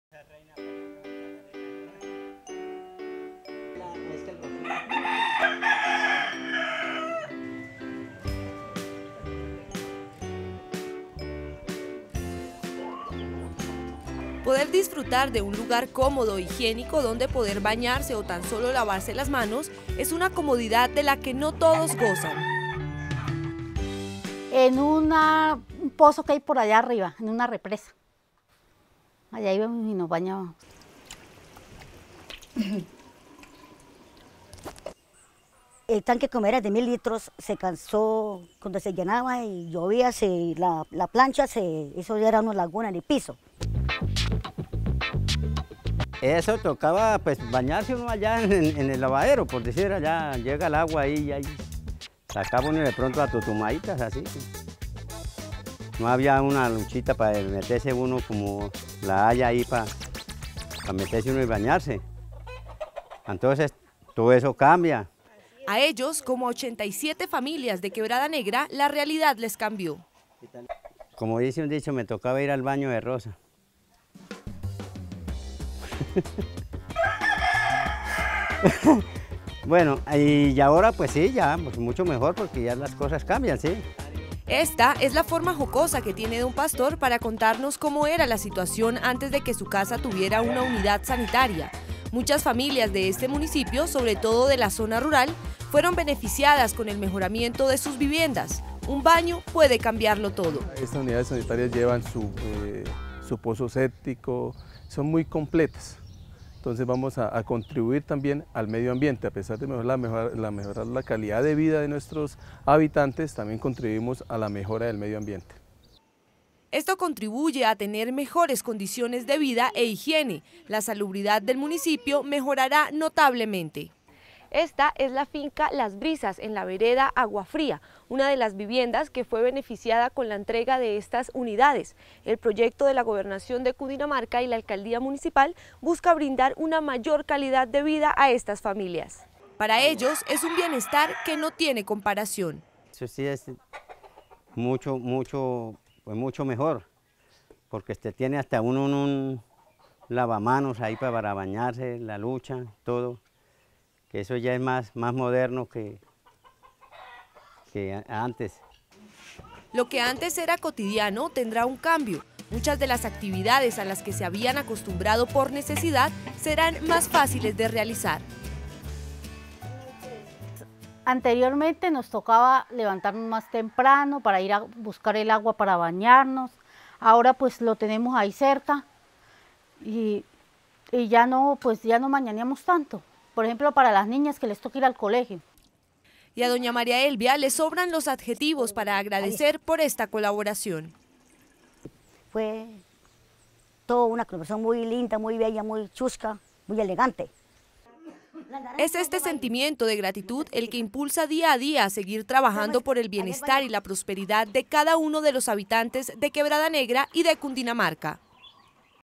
Quebradanegra- Mejoramiento de Viviendas
87 familias del municipio de Quebradanegra se benefician del mejoramiento de sis viviendas. Sus casas cuentan ahora con unidades Sanitarias